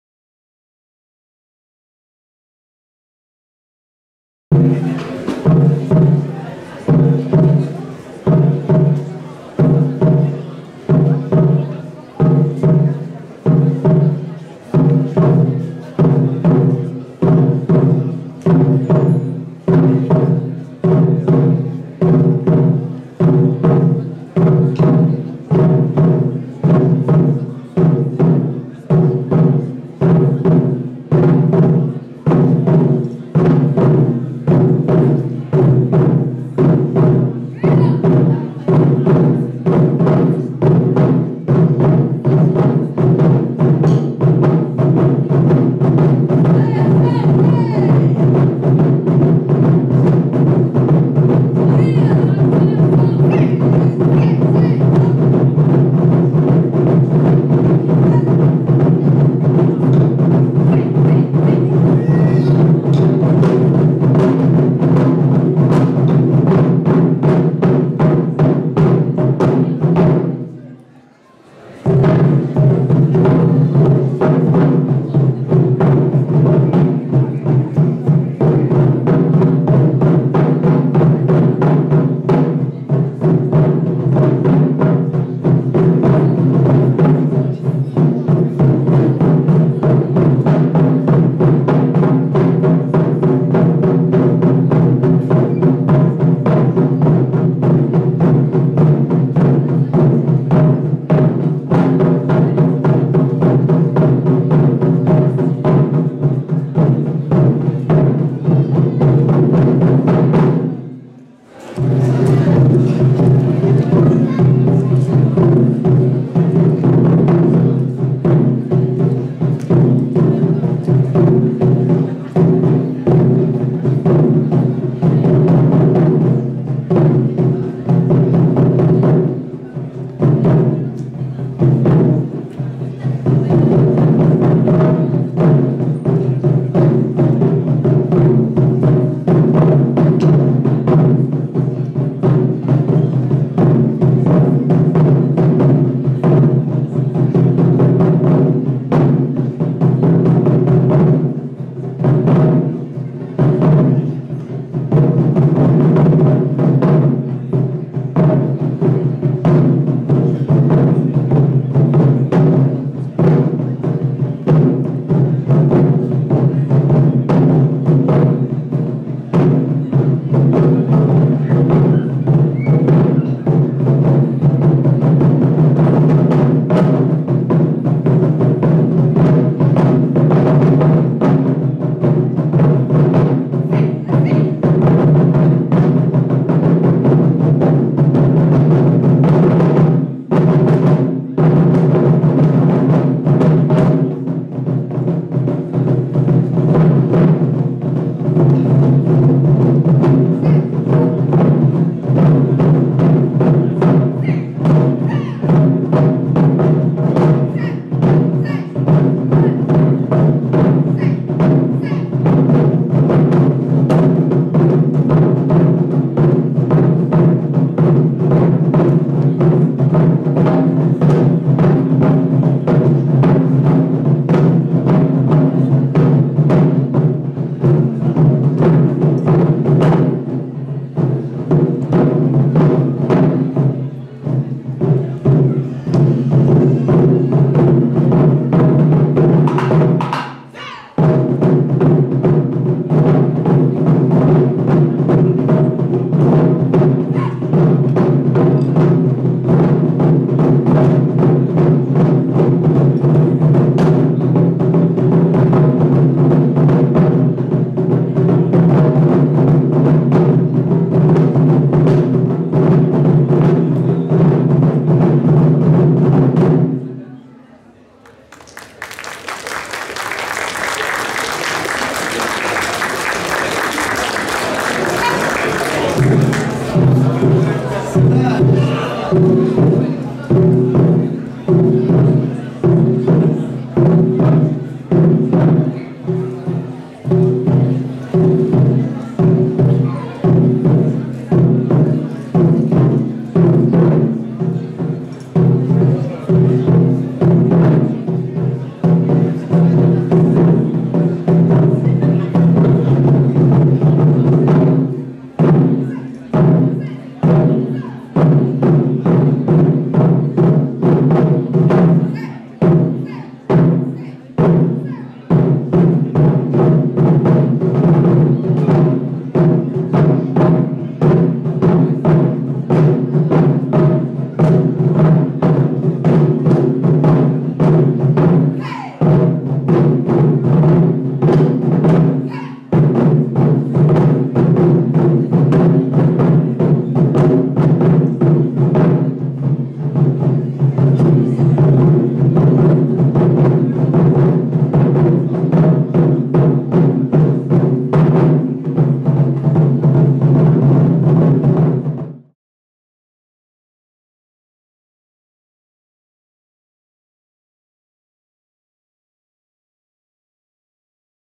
Concert Makoto Cosgeek 2022
Concert du groupe de musique de Makoto a la convention Cosgeek